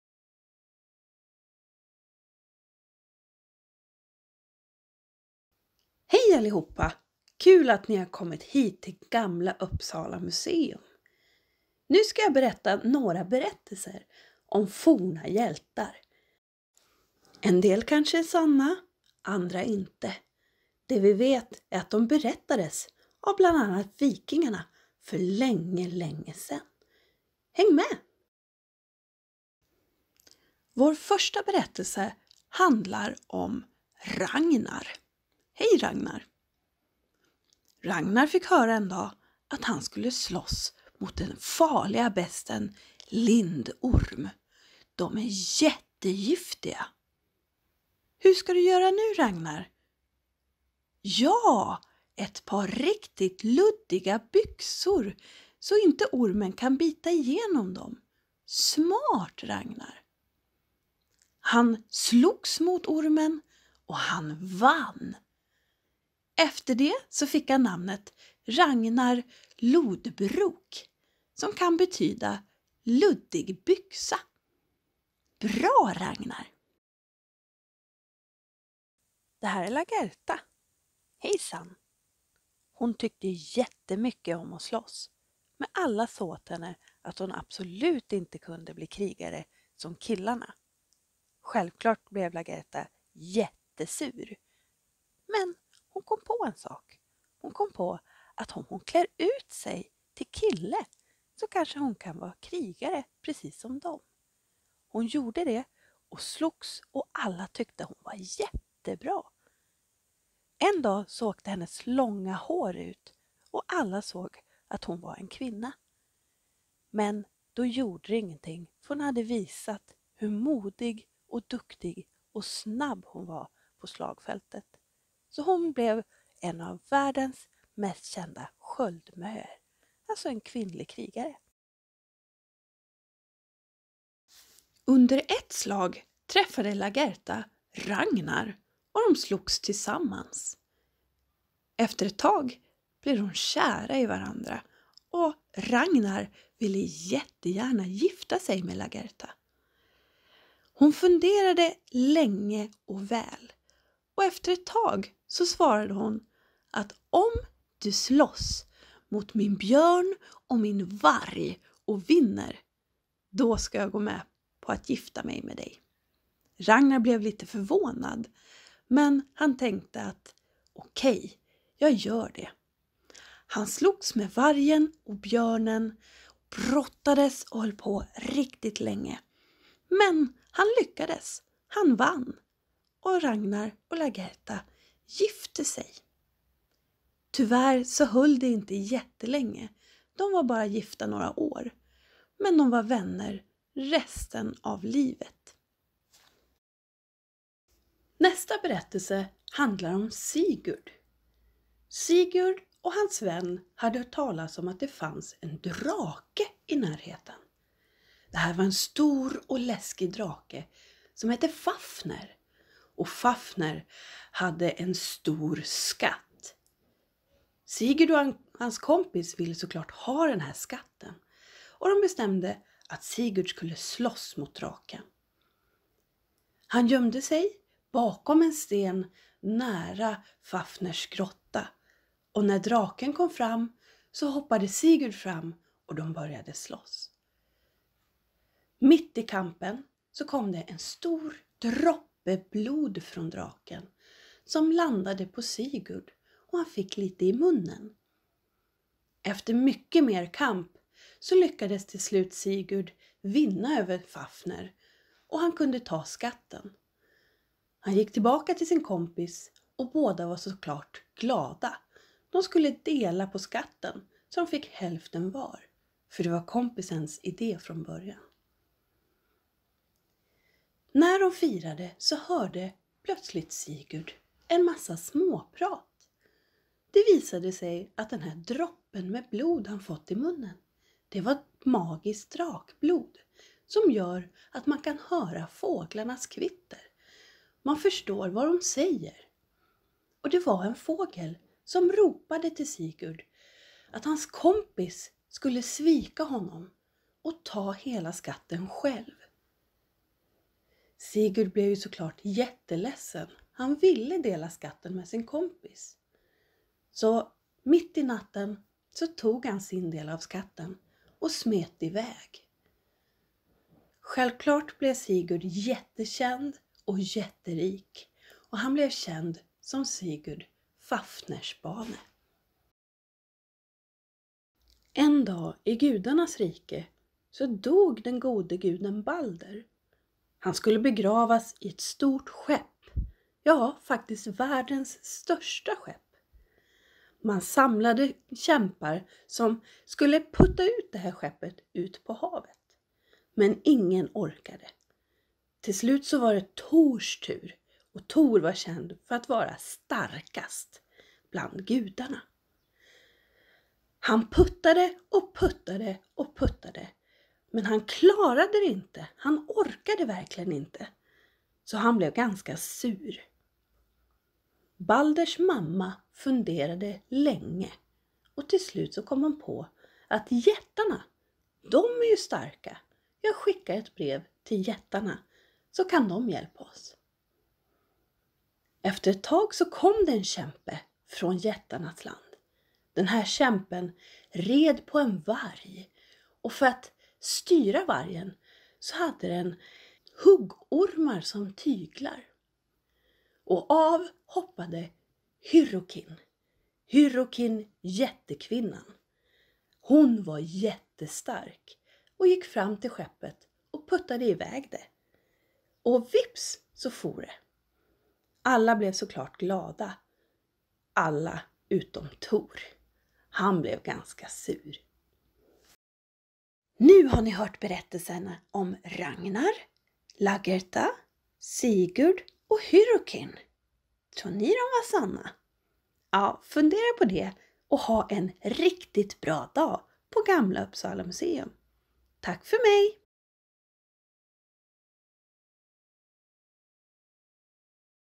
Kämpar från förr
Vad hittar vi för modiga hjältar i vikingarnas berättelser och sagor? Det tar vi reda på i den här filmen. Lämplig för barn i de flesta åldrar.